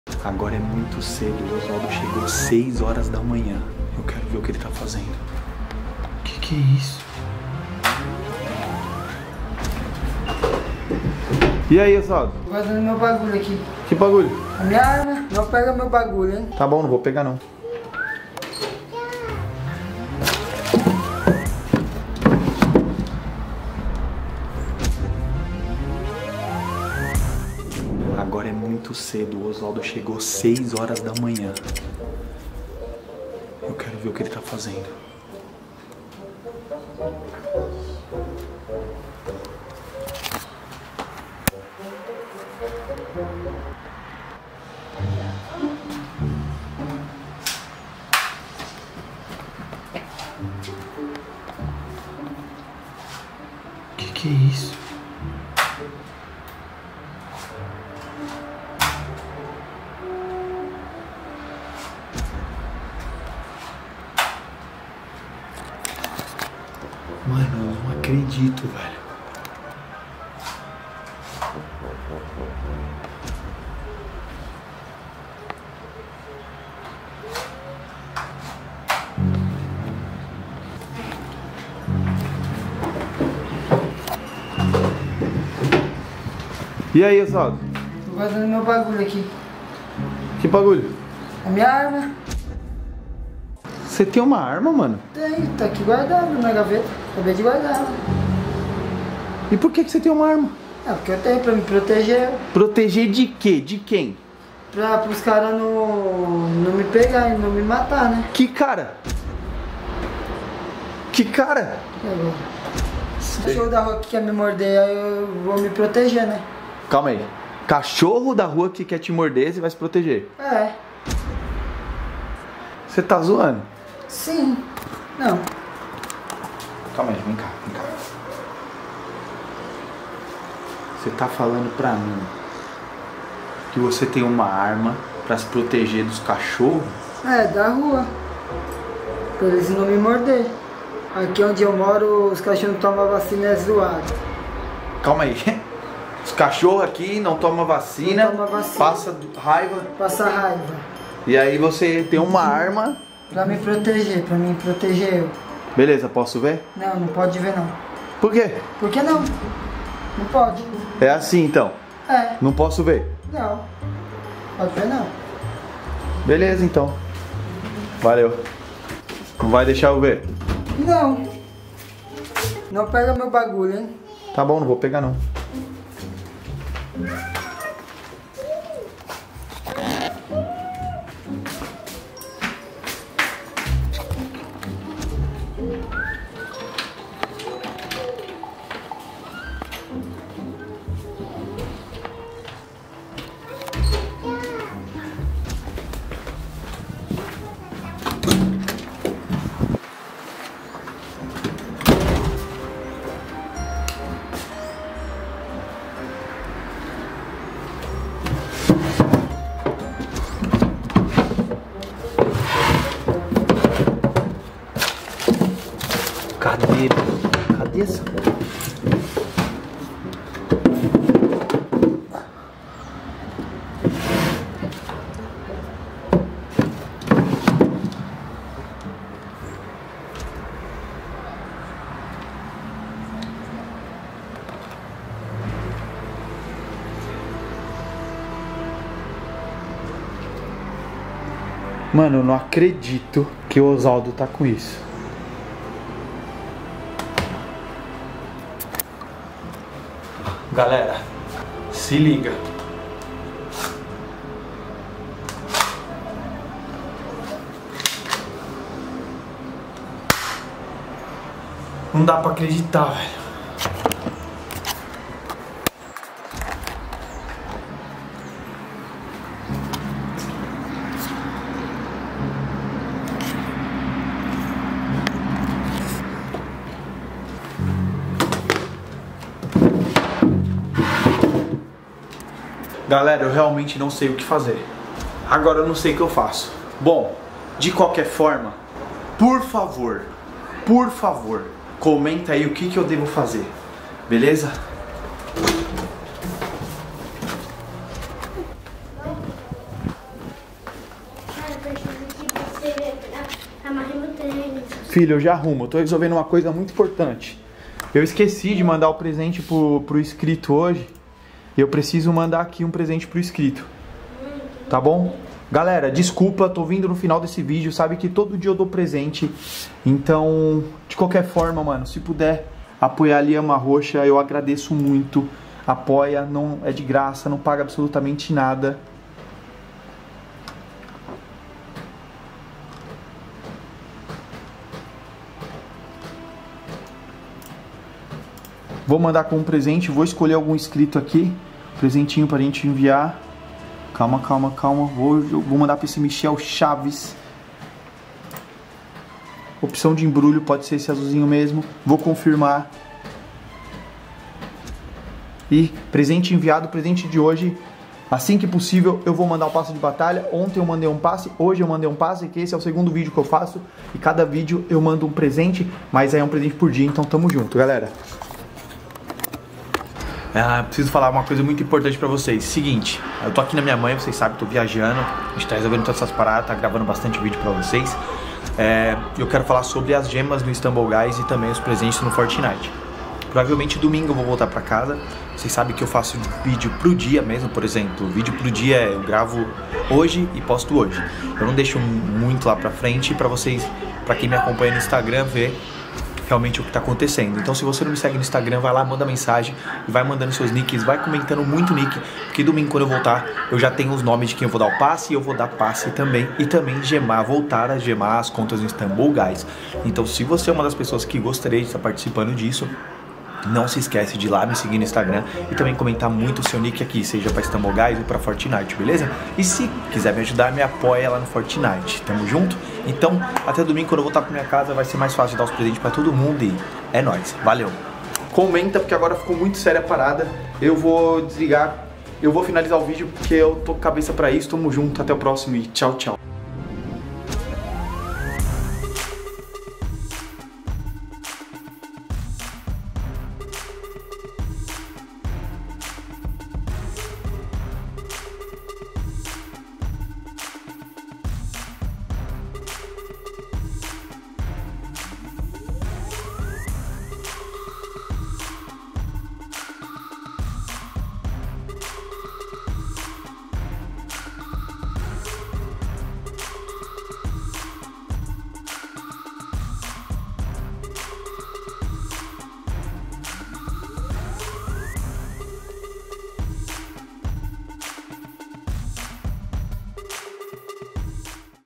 Descobri o Maior Segredo do Meu Irmão, Olha o que ele tem na Casa da Minha Mãe!
oi gente lhama roxa aqui e hoje o video e o seguinte todos sabem os problemas que estamos enfrentando com meu irmao, obviamente estamos tentando o maximo trazer todos os conteudos para voce! porem hoje eu descobri o maior segredo do meu irmao!
lembrando que no video de hoje vai ter presente no fortnite
o osvaldo meu irmao e um bom garoto um cara muito legal, por isso eu amo ele estou com ele em todas as etapas!
oi galera este e o terceiro video da serie da vape e do pod, pois se voce esta chegando agora quero dizer que minha mae me ligou pra dizer que achou uma vape nas coisas do meu irmao uma pod nas coisas dele e eu vim correndo e sim meu irmao esta fumando vape  e pod cigarro eletronico, eu conversei com ele, e ele confessou o problema foi que no video de hoje o braian viu ele o irmao do lhama roxa vendendo vape, e me falou e o lhama roxa foi correndo la  e pegou ele vendendo vape, o problema foi que o lima rocha achou varias vapes e pod recarregavel.
e agora eu vou conta pra minha mae, pra entender o porque ele esta fazendo isso!
tag unica!
ajfuhsuijiqijihiwfhdifhi lhama roxa é o melhor